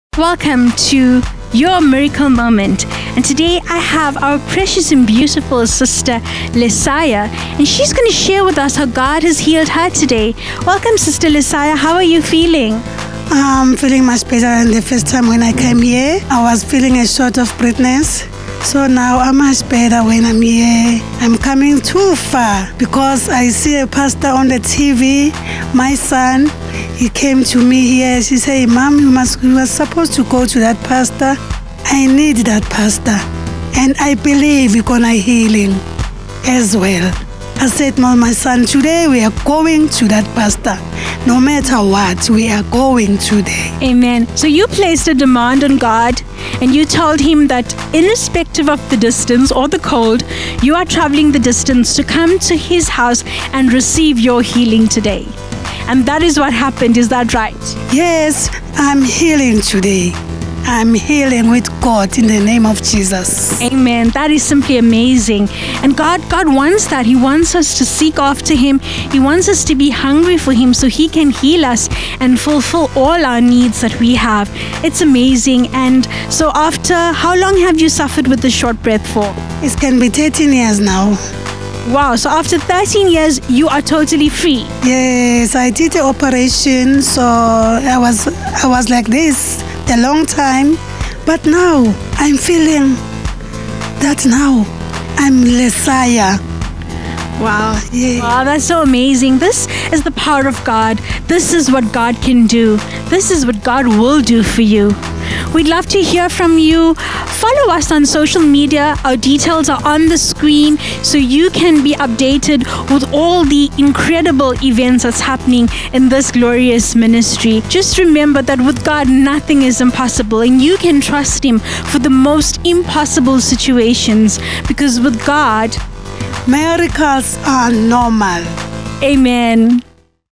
Healed from shortness of breath after 13 years! │Miracle Report with Katy
Healed from shortness of breath after 13 years

Lesiah received her healing from shortness of Breath after 13 years. She is totally free.